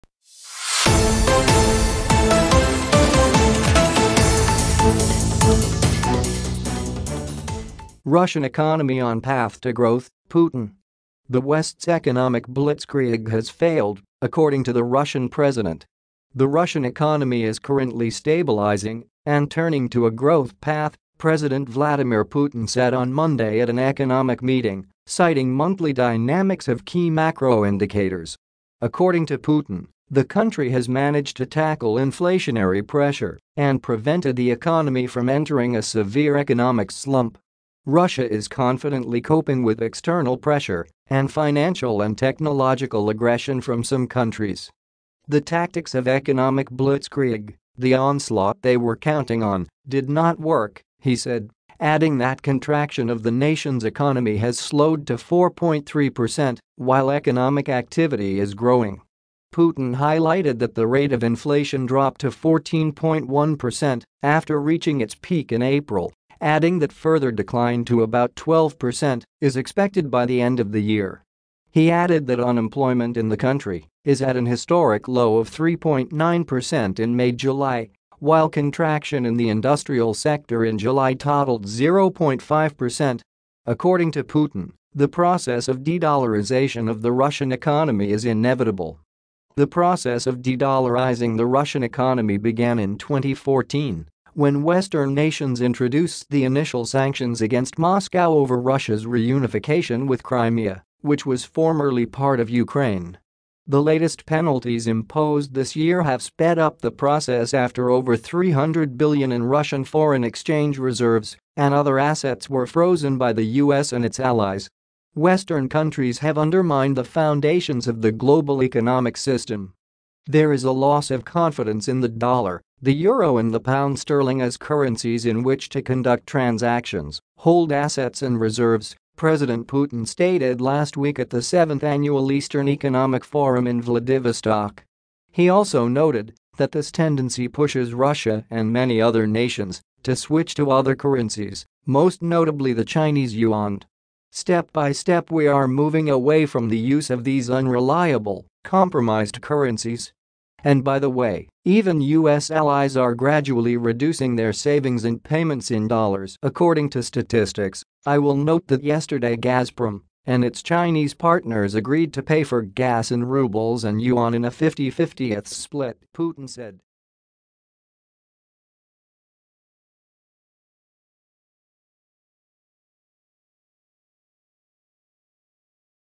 Russian economy on path to growth – Putin.
The West’s economic ‘blitzkrieg’ has failed, according to the Russian president.
The Russian economy is currently stabilizing and turning to a growth path, President Vladimir Putin said on Monday at an economic meeting, citing monthly dynamics of key macro indicators.

According to Putin, the country has managed to tackle inflationary pressure and prevented the economy from entering a severe economic slump.

“Russia is confidently coping with external pressure, and financial and technological aggression from some countries. The tactics of economic blitzkrieg, the onslaught they were counting on, did not work,” he said, adding that contraction of the nation’s economy has slowed to 4.3%, while economic activity is growing.

Putin highlighted that the rate of inflation dropped to 14.1% after reaching its peak in April, adding that further decline to about 12% is expected by the end of the year. He added that unemployment in the country is at an historic low of 3.9% in May-July, while contraction in the industrial sector in July totalled 0.5%.

According to Putin, the process of de-dollarization of the Russian economy is inevitable.
The process of de-dollarizing the Russian economy began in 2014, when Western nations introduced the initial sanctions against Moscow over Russia’s reunification with Crimea, which was formerly part of Ukraine. The latest penalties imposed this year have sped up the process after over $300 billion in Russian foreign exchange reserves and other assets were frozen by the US and its allies.

“Western countries have undermined the foundations of the global economic system. There is a loss of confidence in the dollar, the euro and the pound sterling as currencies in which to conduct transactions, hold assets and reserves,” President Putin stated last week at the seventh annual Eastern Economic Forum in Vladivostok.

He also noted that this tendency pushes Russia and many other nations to switch to other currencies, most notably the Chinese yuan.

“Step by step we are moving away from the use of these unreliable, compromised currencies. And by the way, even US allies are gradually reducing their savings and payments in dollars, according to statistics... I will note that yesterday Gazprom and its Chinese partners agreed to pay for gas in rubles and yuan in a 50/50 split,” Putin said.